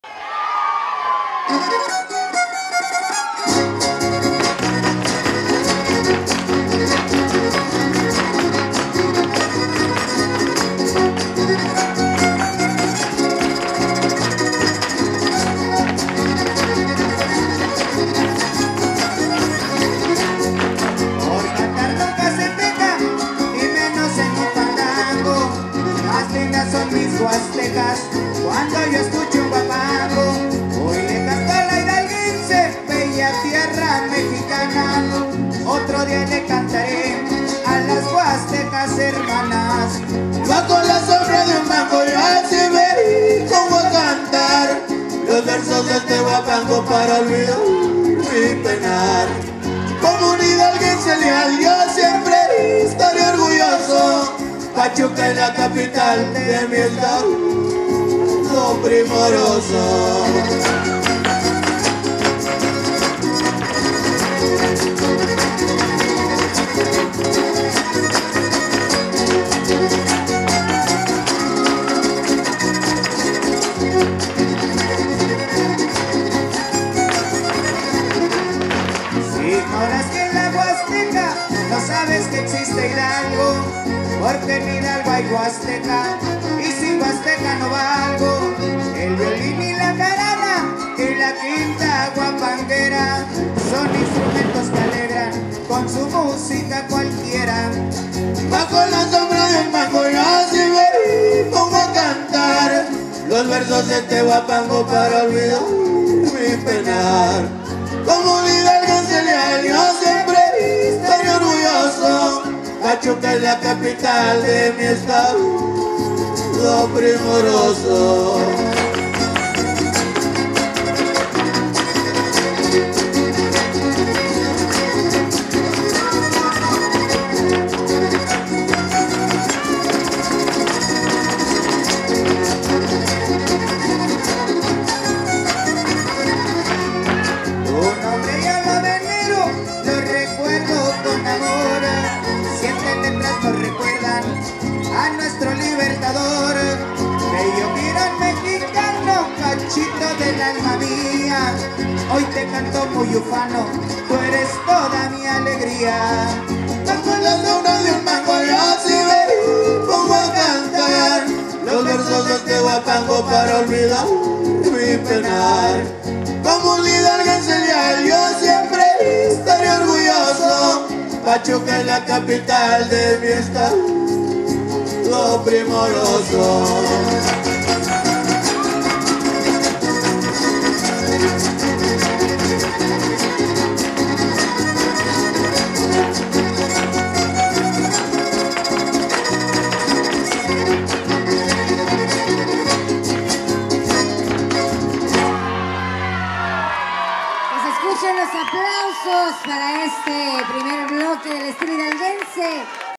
Huasteco Hidalguense con el Trío Los Hidalguenses en Nicolás Flores, Hidalgo 2024 🎻🎻🎻
Huasteco Hidalguense con el Trío Los Hidalguenses presentes en el XL Concurso Nacional de Huapango Profesional en Nicolás Flores, Hidalgo 2024 🎻🎻🎻
Así se Vive el Huapango con el trío